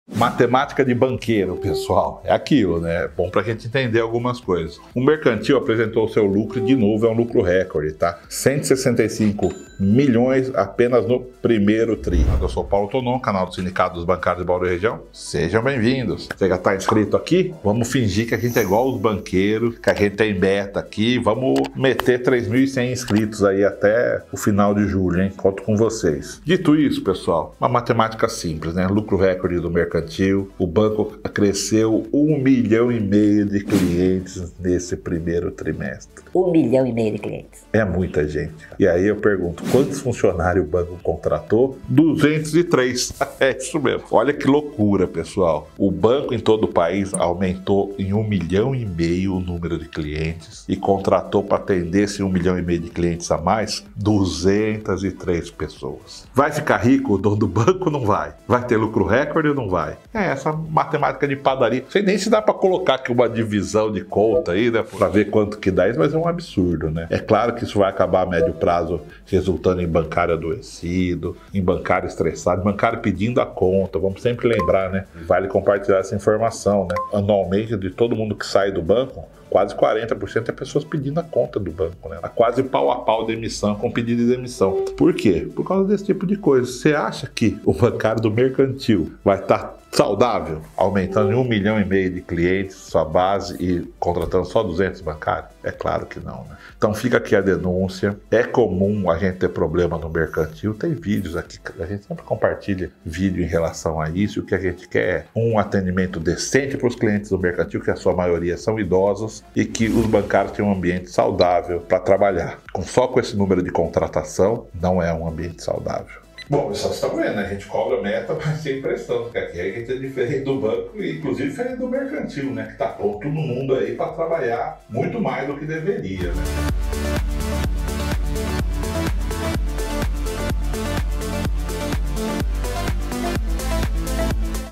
LUCRO RECORDE: MUITOS NOVOS CLIENTES E POUCAS CONTRATAÇÕES DE BANCÁRIOS
No vídeo de hoje, o diretor de imprensa do Sindicato dos Bancários de Bauru e Região, Paulo Tonon, fala sobre o lucro recorde do Mercantil no primeiro trimestre de 2024, quando houve um aumento de cerca de 1,5 milhão de novos clientes para serem atendidos por aproximadamente 200 novos contratados. A conta não fecha!

Veja e tire suas próprias conclusões. Publicamos vídeos novos todos os dias, sempre em defesa dos direitos dos trabalhadores e contra a ganância dos patrões!